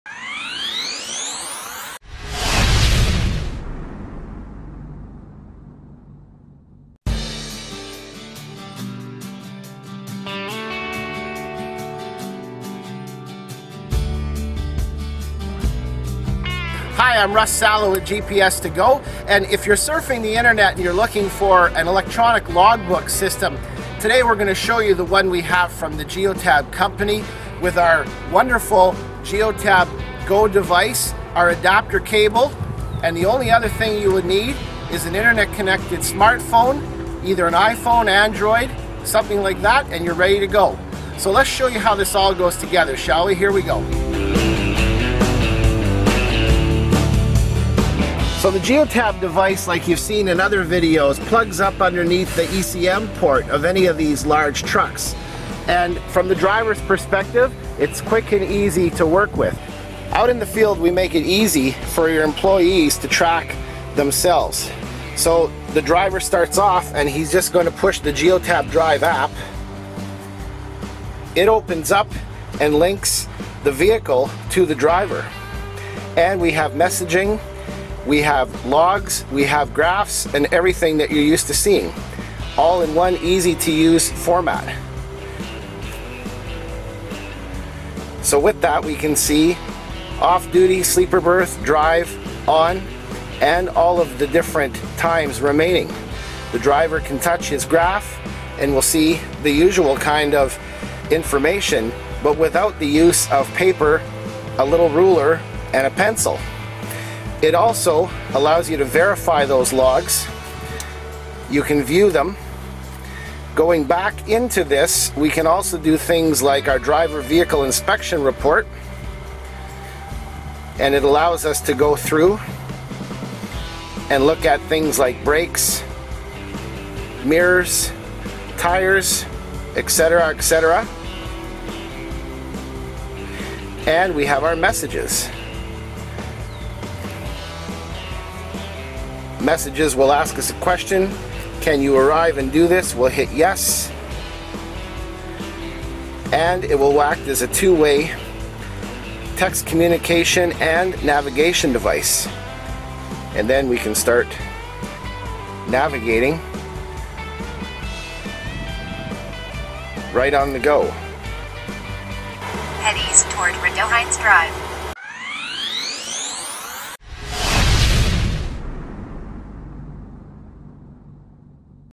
GPS to GO Electronic Logging Devices Help FMCSA 395.15 Compliance
No need to spend thousands to get world class electronic log books! The Geotab telematics system is used by 80% of Canadian and USA Fleets over 500 vehicles & we have a fully compliant FMCSA 395.15 electronig log package you can run using our plug-and-play GPS device along with any Android or Apple smartphone or connected tablet.

No contracts! No major hardware & set-up fees!